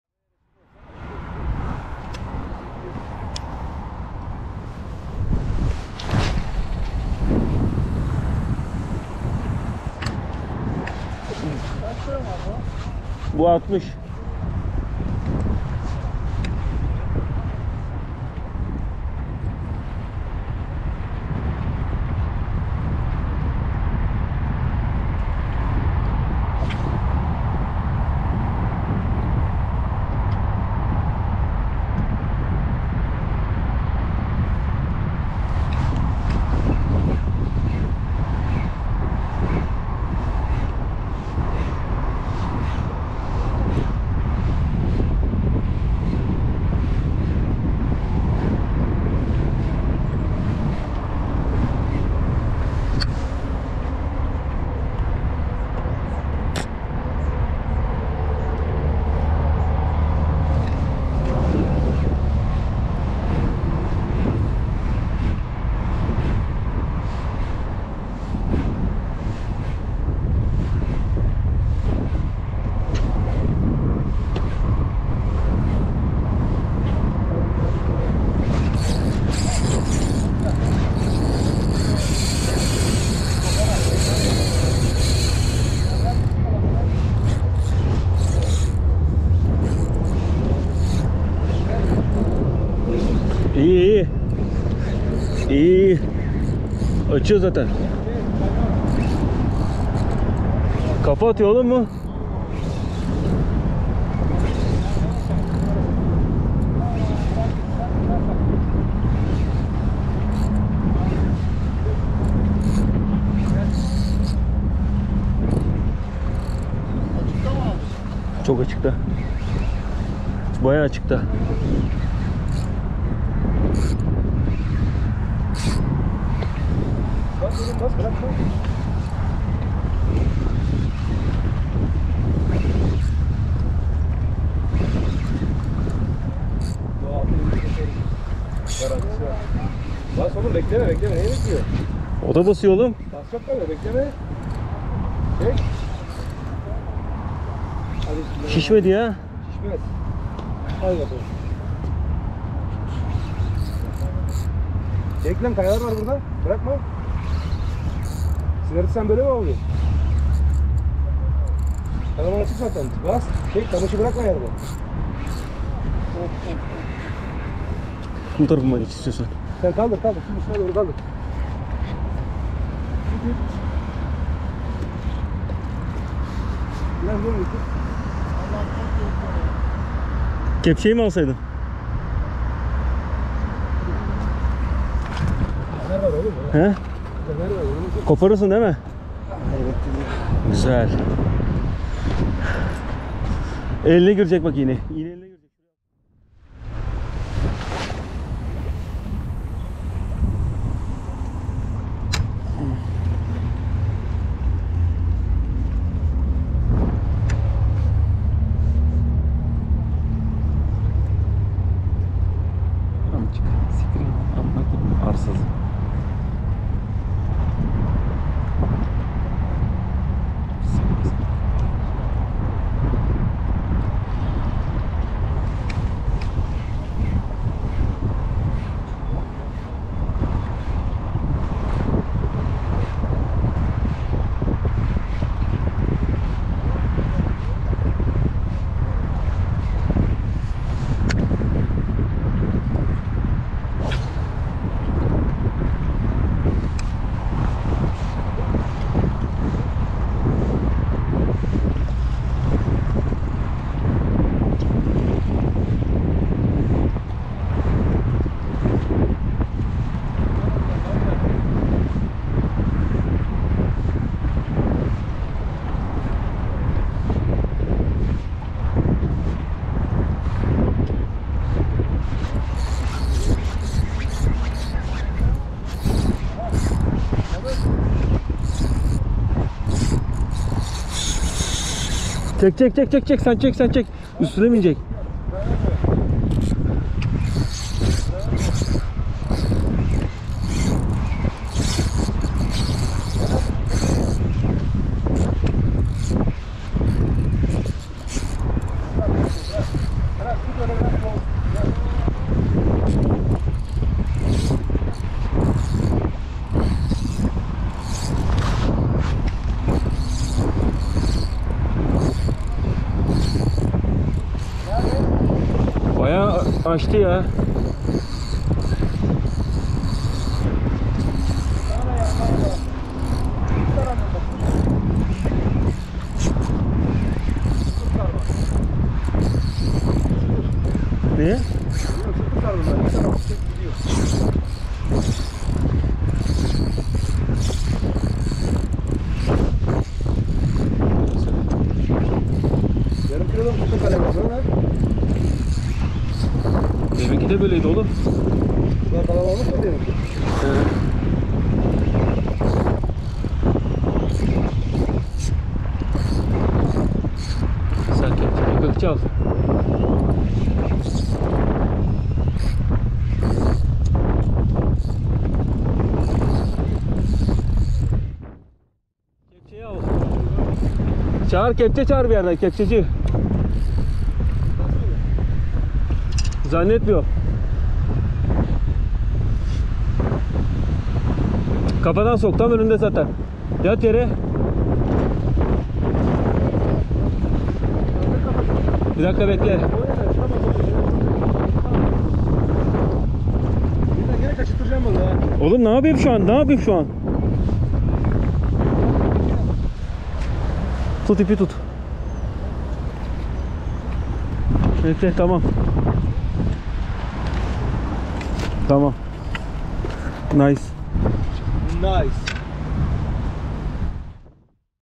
Granyöz Avı İzmir ( yarım saat içinde iki granyöz )
granyöz avımız öğlen vakti yarım saat içinde gelen iki güzel granyöz